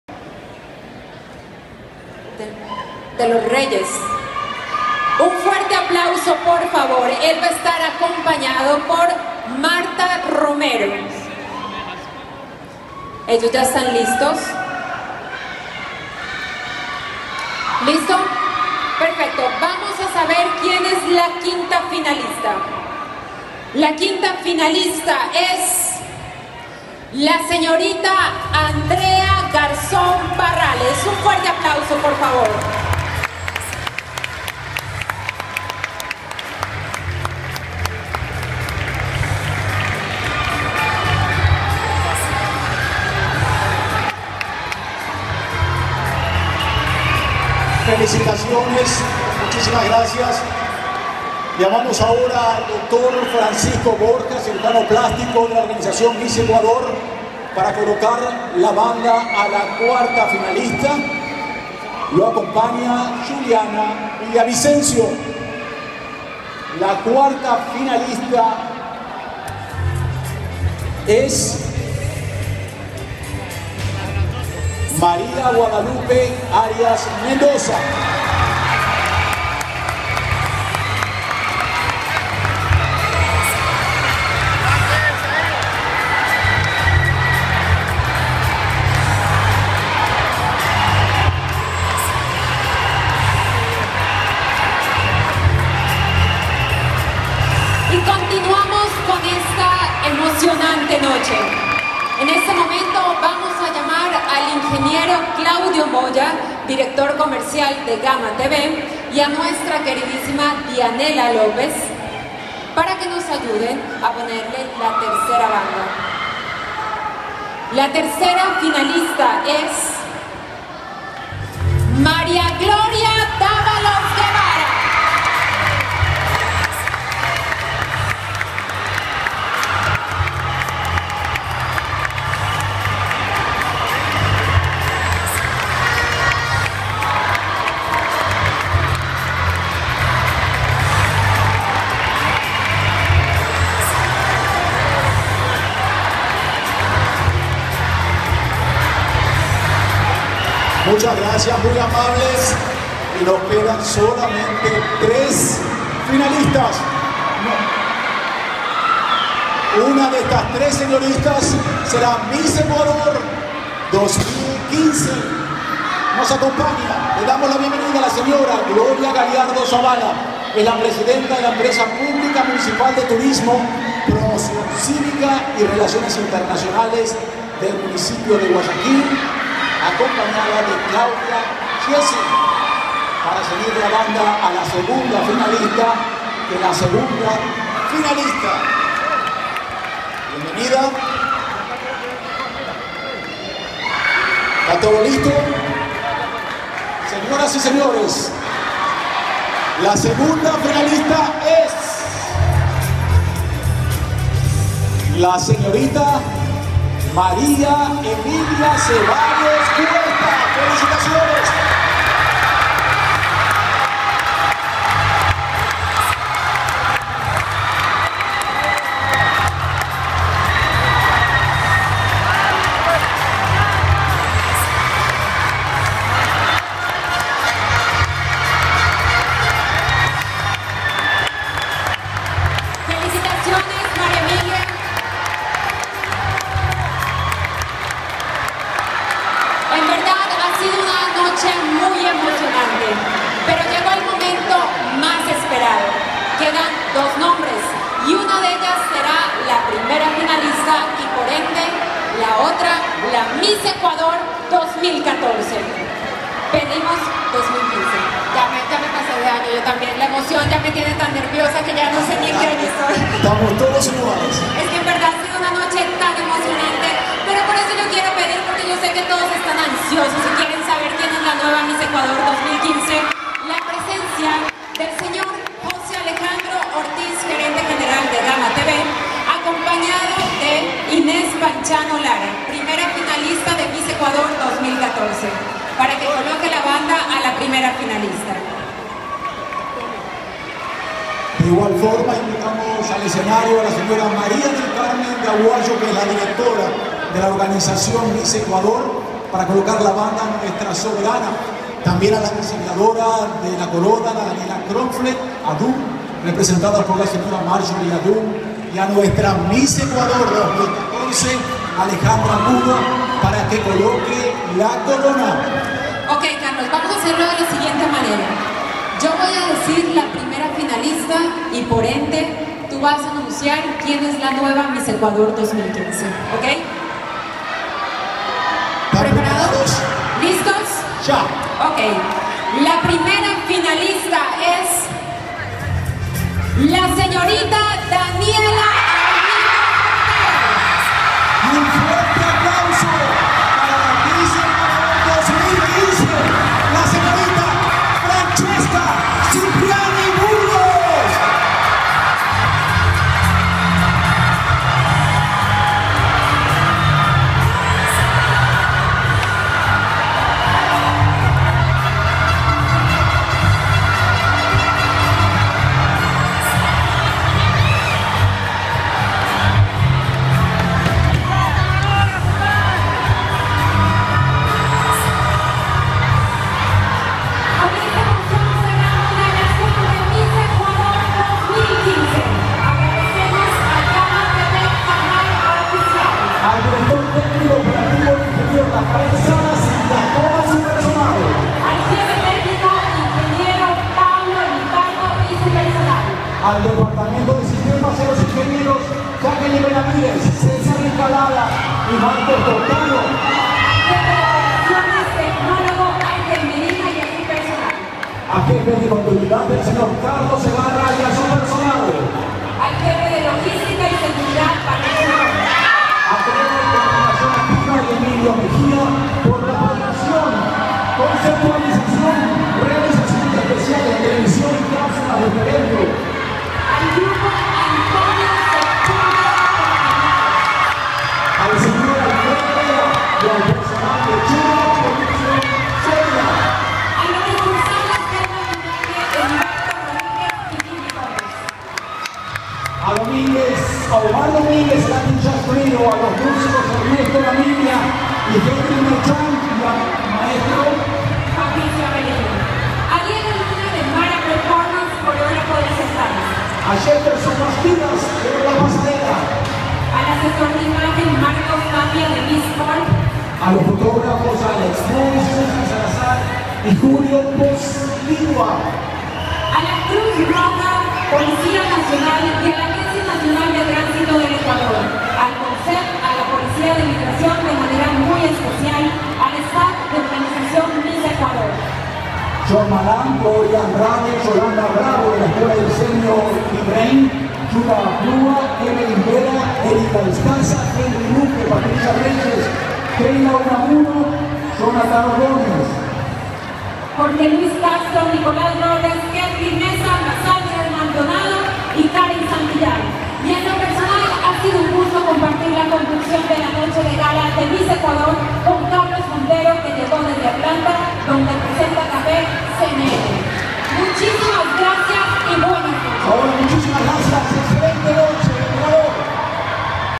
Francesca Cipriani es la nueva Miss Ecuador 2015.
De entre 18 bellas jóvenes, la guayaquileña Francesca Cipriani, de 21 años, fue elegida Miss Ecuador 2015, en la sexagésima edición del certamen. La gala se realizó este sábado 14 de marzo en el Centro de Convenciones Simón Bolívar de Guayaquil.